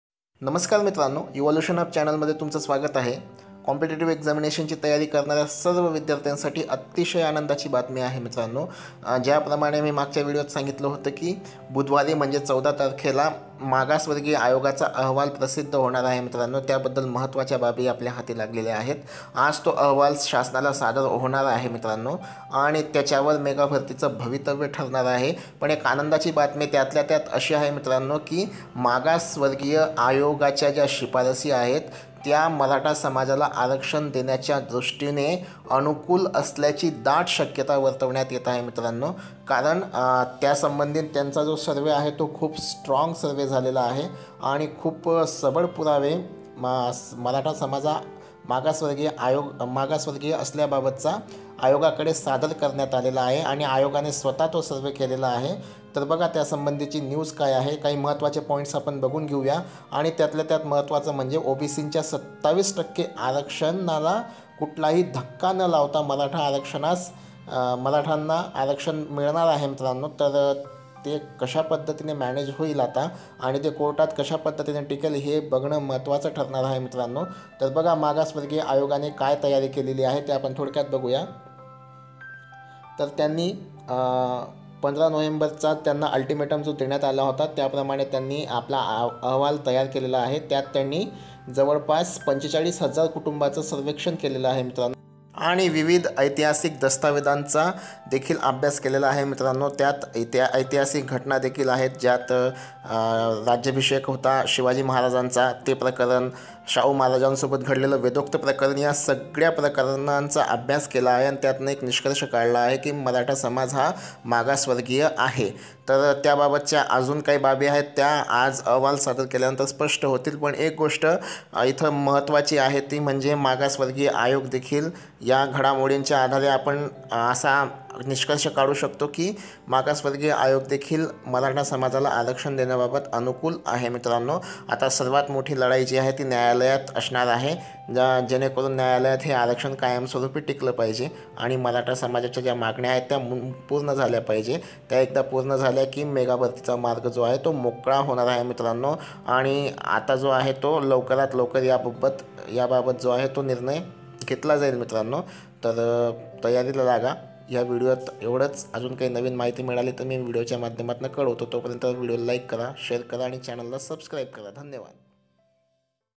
मेगा भरती लवकरच होणार | आज मेगा भरती चा निर्णय होणार
मित्रांनो मागासवर्गीय आयोगाचा अहवाल शासनाला सादर व्हायला जात आहे त्यात मेगा भरतीच भवितव्य आणि मराठा आरक्षणाच भवितव्य ठरणार आहे मित्रांनो संबंधित वृत्त व्हिडिओमध्ये